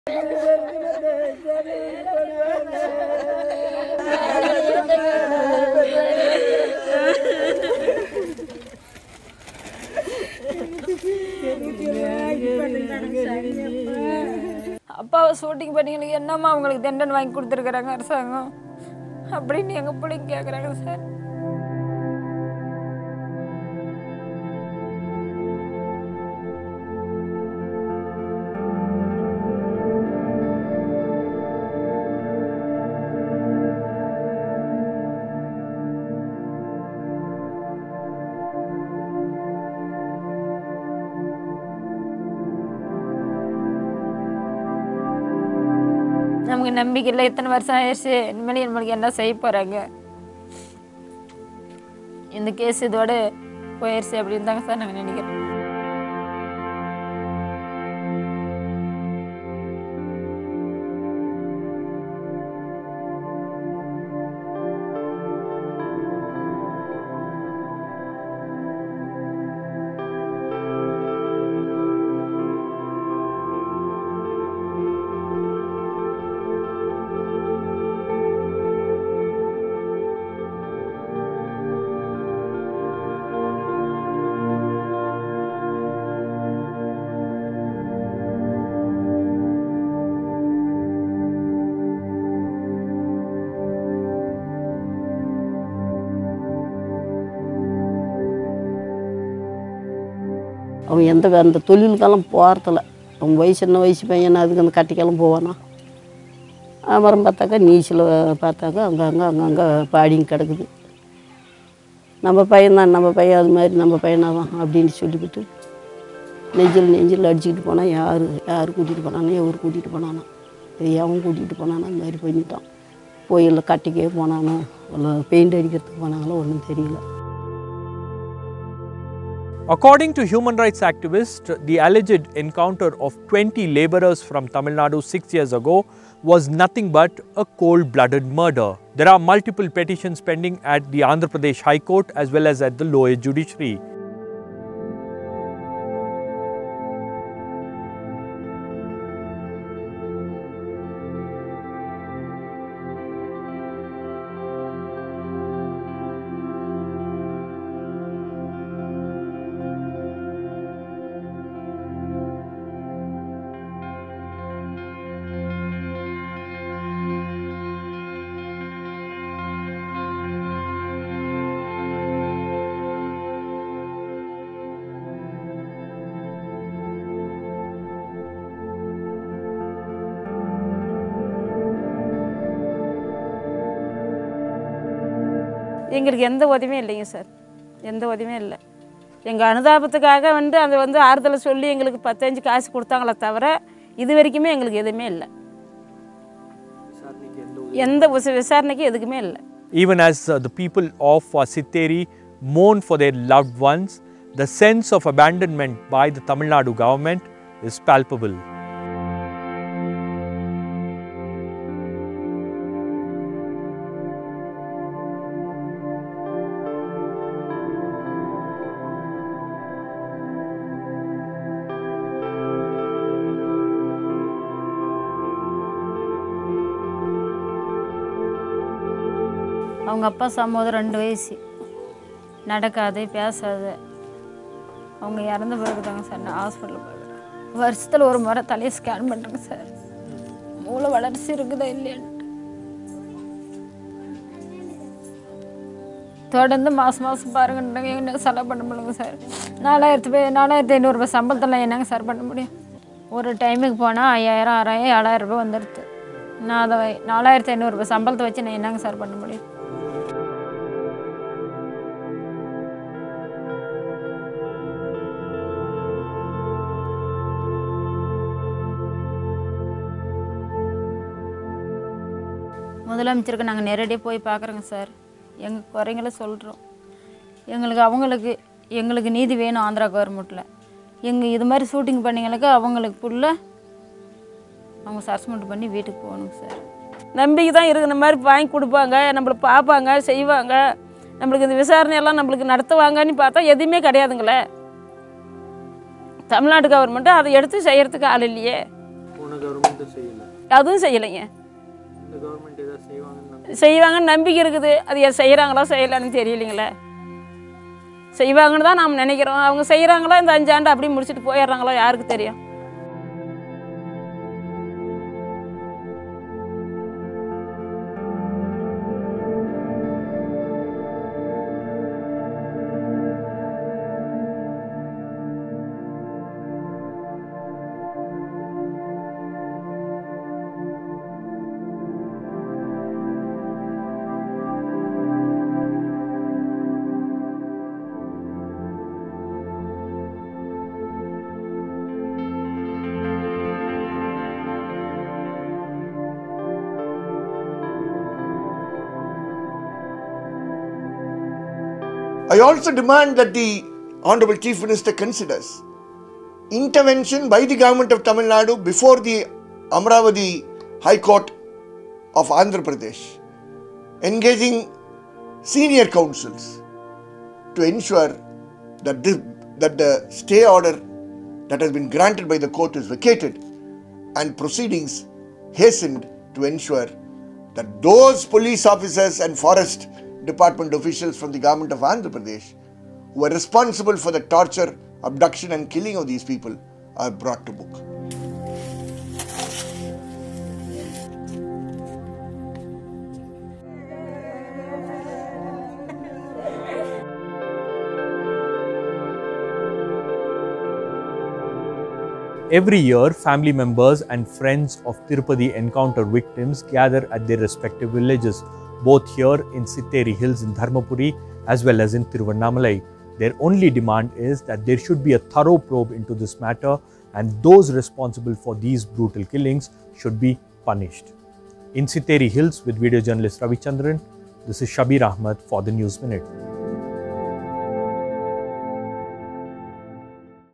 20 men from Tamil Nadu shot dead in 2015, no justice yet
7 years on, justice eludes families of 20 men killed by Andhra police in Seshachalam forest.

The 20 men from Tamil Nadu were shot dead by the Andhra Pradesh Red Sanders Anti-smuggling Special Task Force, who alleged that they were part of the racket to fell and smuggle red sanders. However, the families of the victims say the men were only daily wage workers and that they were killed in a ‘fake encounter’.

While police officials involved in the alleged extrajudicial killings are yet to be brought to book,  families allege that despite promises they have not received any legal support from the Tamil Nadu government and feel they have been abandoned by the state.